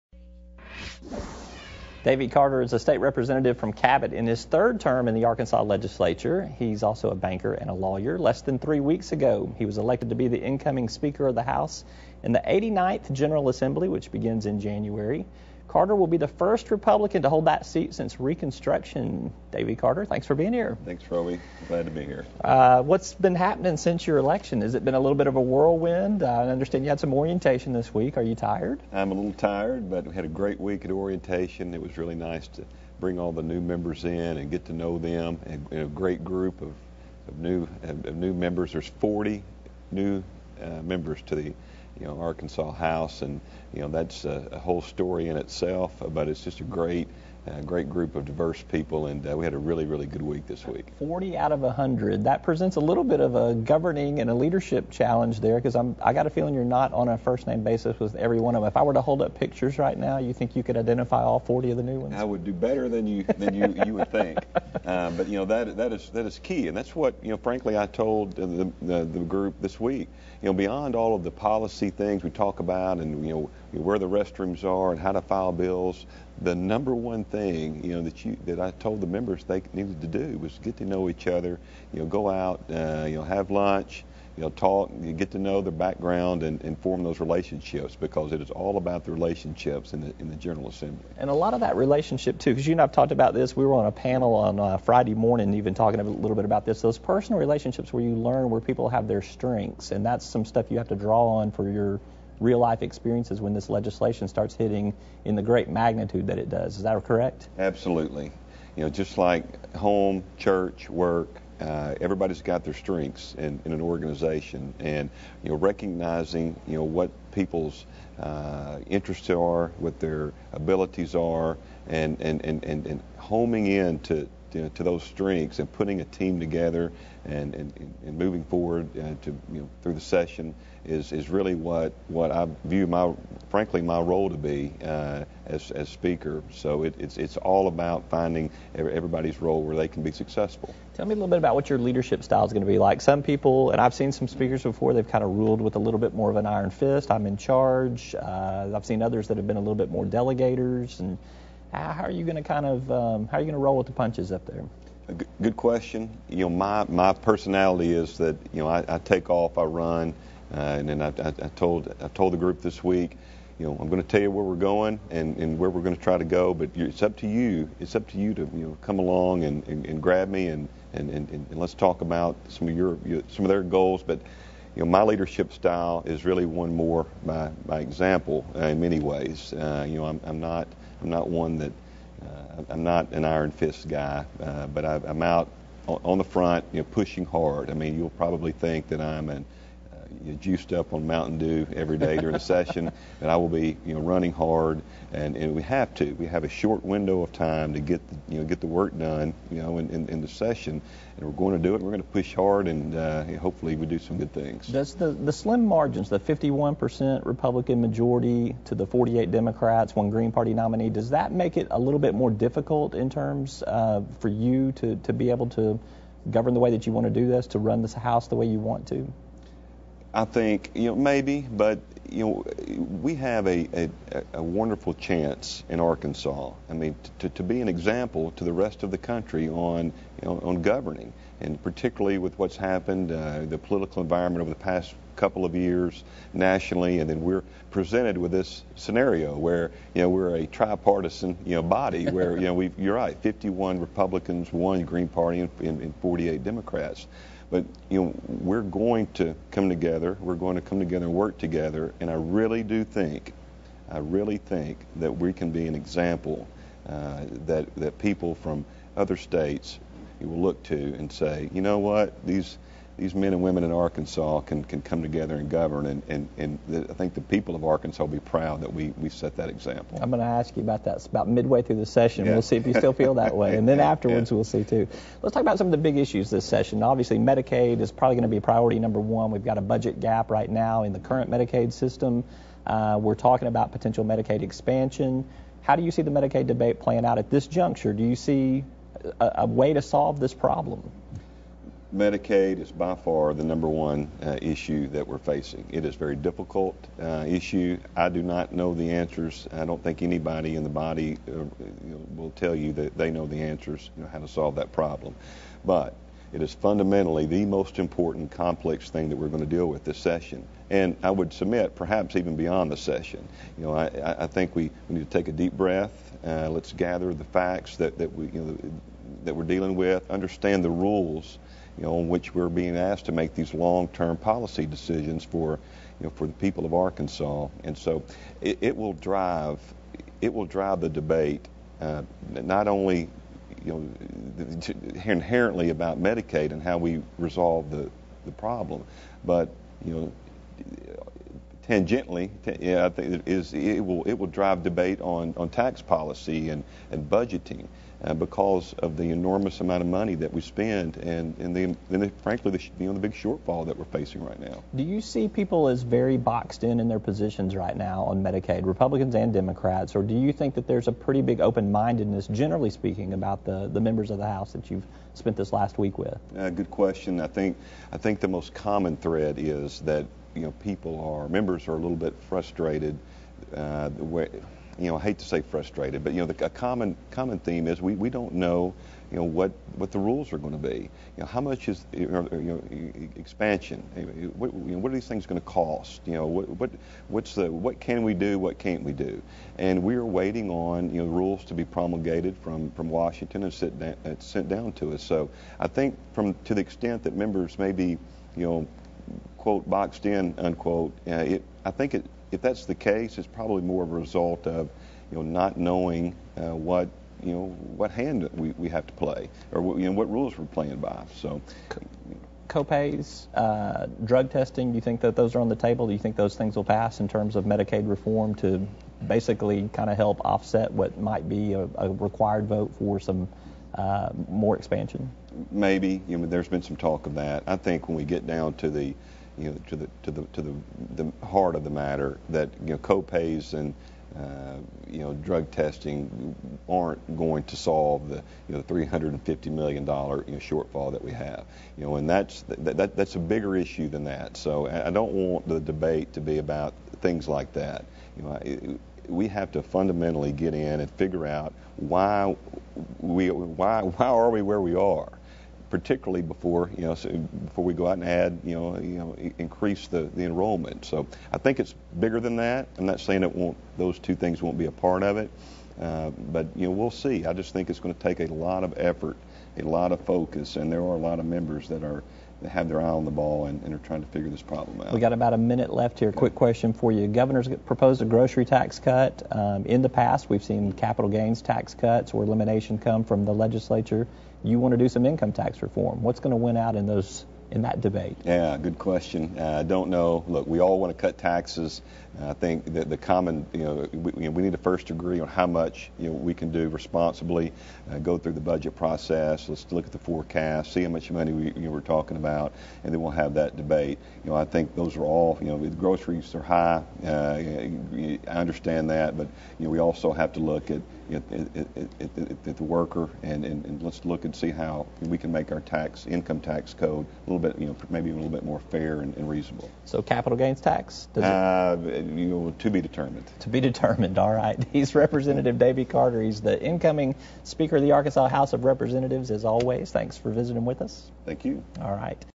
DavyCarter
House Speaker-elect Davy Carter (R-Cabot) discusses his leadership style and the big issues facing the 89th General Assembly.